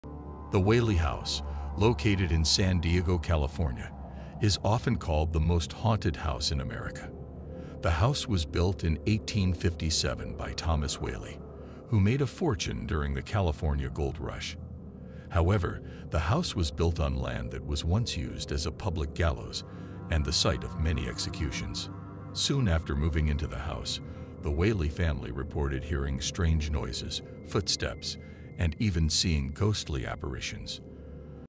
Whaley House
Discover the chilling history of the Whaley House, known as one of the most haunted places in America. Learn about the tragic events that occurred within its walls and the spirits that are said to still roam its halls. Join us on a spine-tingling journey through this notorious haunted house.